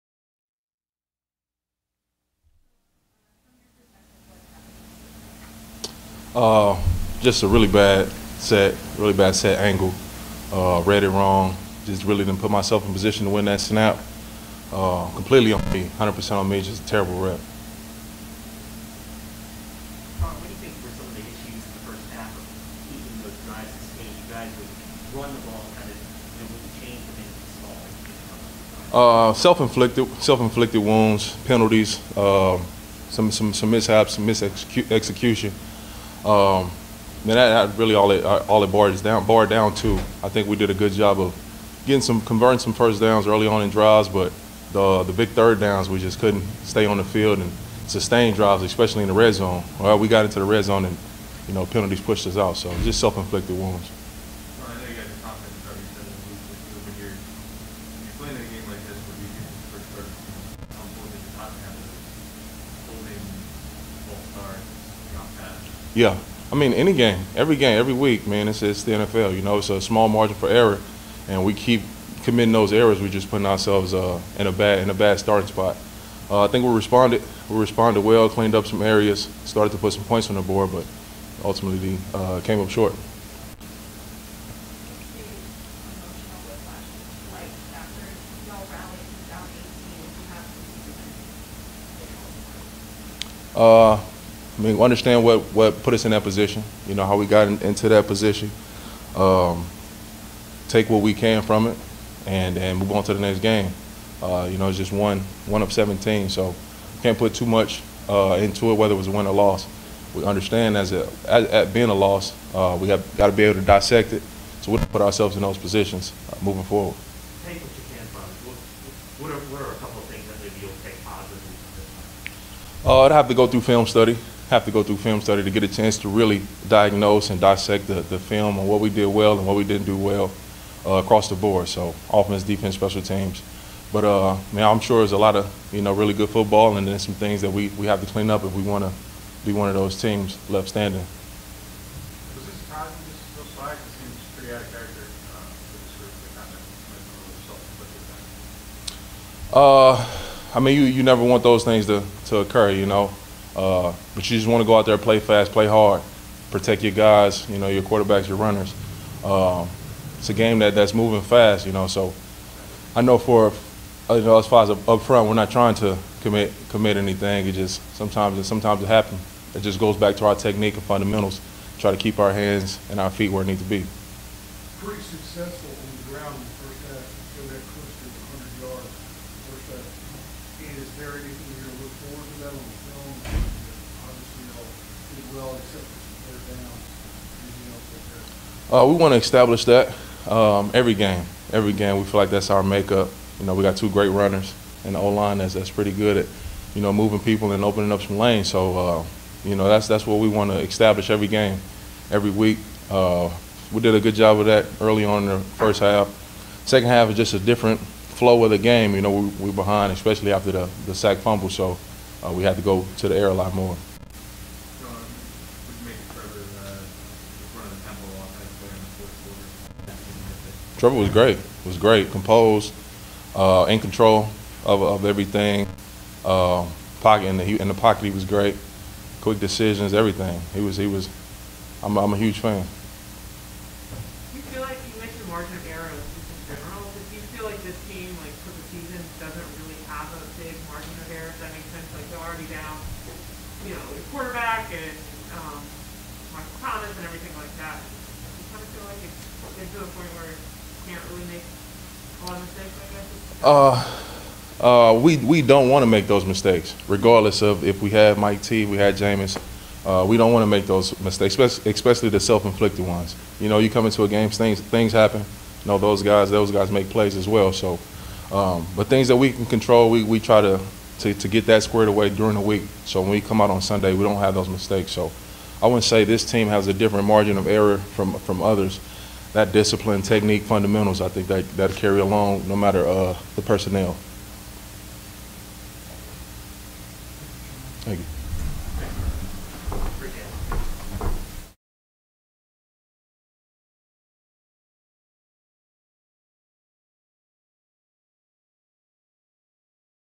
Terron Armstead on Protection vs. Atlanta | Saints-Falcons Postgame
New Orleans Saints left tackle Terron Armstead postgame interview following the saints loss to the Atlanta Falcons in week 9 of the 2021 NFL season. Saints vs Falcons Game Center